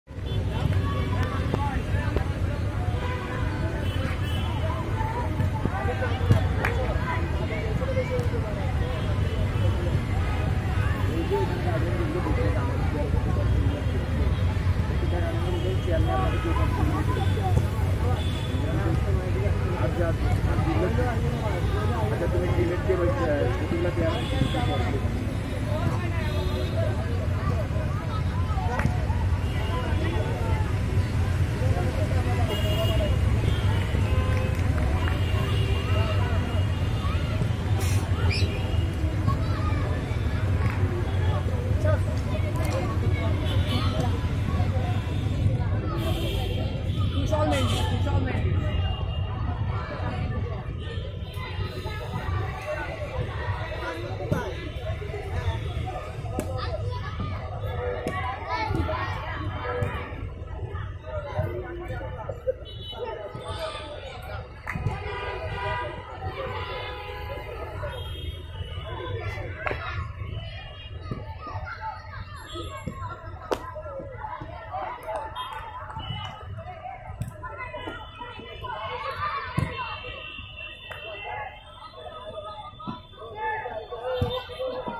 এঁরাই বাংলাদেশকে বিশ্বকাপ ট্রফি এনে দিবে⚽🏃‍♂️🏃‍♀️📢🎉🇧🇩🌎
এরাই আগামী দিনের ভবিষ্যৎ এখান থেকে উঠে আসবে বাংলার মেসি,নেইমার ও এমবাপ্পে মতো তারকারা⚽🏃‍♂️🏃‍♀️📢💚🎉🇧🇩🌎 anissur rahman